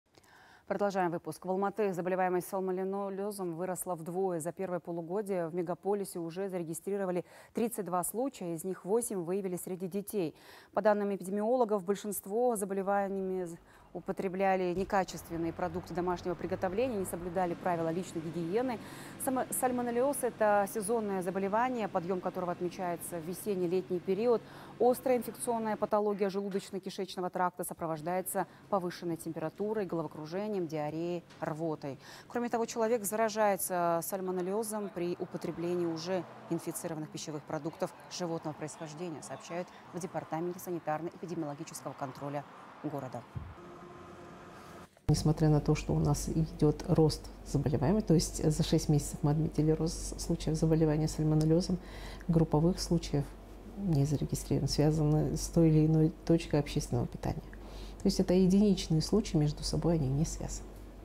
Сезонное заболевание: санврачи предупредили алматинцев
В Алматы заболеваемость сальмонеллёзом выросла вдвое. За первое полугодие в мегаполисе уже зарегистрировано 32 случая, из них 8 выявили среди детей. По словам эпидемиологов, большинство заболевших употребляли некачественные продукты домашнего приготовления, не соблюдали правила личной гигиены. Сальмонеллёз – это сезонное заболевание, подъем которого отмечается в весенне-летний период. Острая инфекционная патология желудочно-кишечного тракта сопровождается повышенной температурой, головокружением, диареей и рвотой. Кроме того, человек заражается сальмонеллезом при употреблении уже инфицированных пищевых продуктов животного происхождения, сообщили в Департаменте санитарно-эпидемиологического контроля города